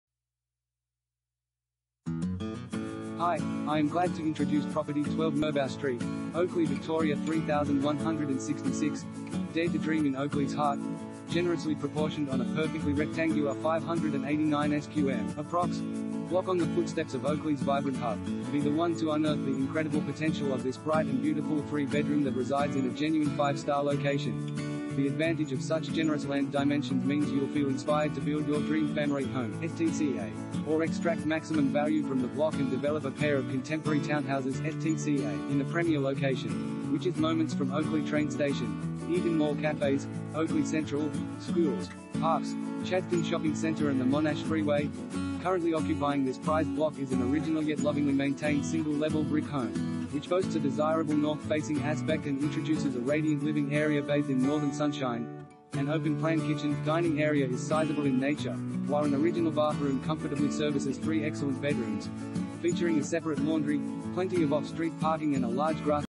12 Merbow Street, Oakleigh VIC 3166 | AuPropertyReport.Com
12 Merbow Street, Oakleigh VIC 3166
Property Type: house
Bedrooms: 3
Bathrooms: 3
Car Space: 1
Dare To Dream In Oakleigh’s Heart